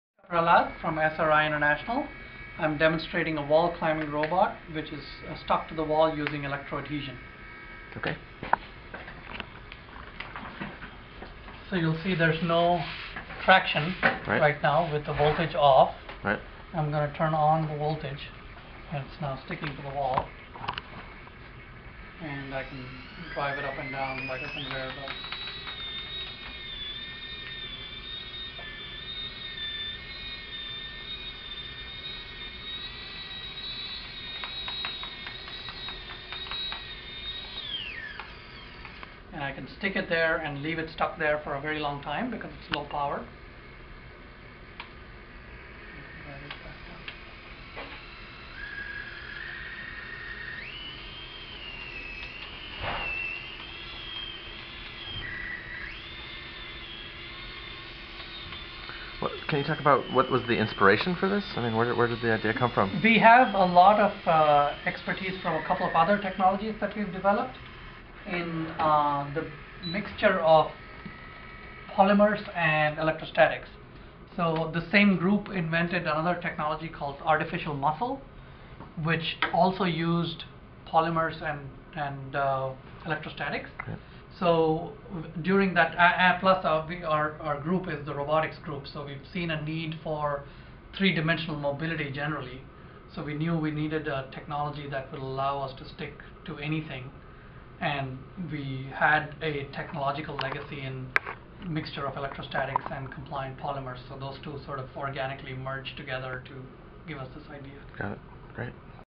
Wall climbing robot
At SRI International, in Menlo Park, California, researchers have developed a robot that is capable of climbing up almost any kind of wall, smooth, flat, or otherwise. Here, Harsha Prahlad, an SRI research engineer, explains the system, which operates under a principle known as electroadhesion.